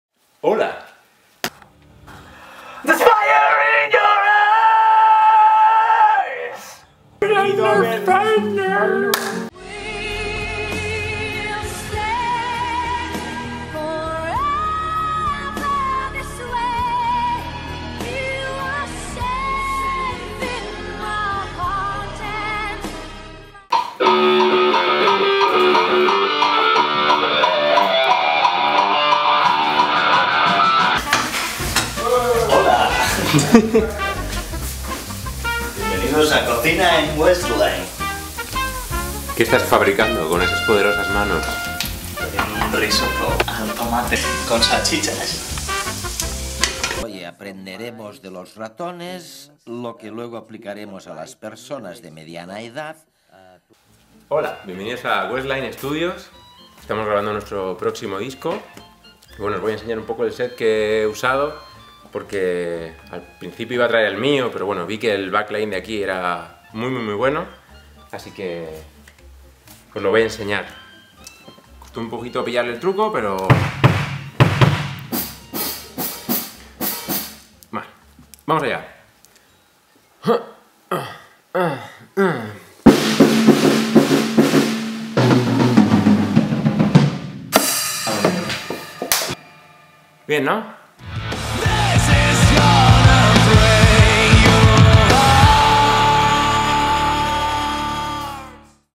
Lamprologus at Westline Studios: take one
Hard working on the studio... Enjoy!

Lamprologus new album:
        Recorded by Juan Blas at Westline studios.
        Mastered by Victor García at Ultramarinos Mastering.

Video: Luis Lopez Pinto